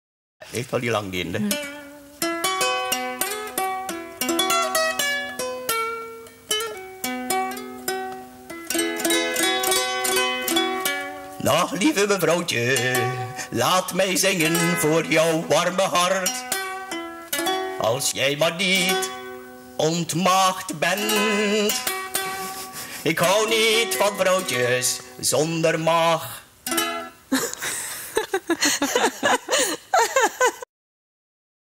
Ygor en miss Belgiê Laura Beyne
Ygor zingt een heel kort liedje voor miss België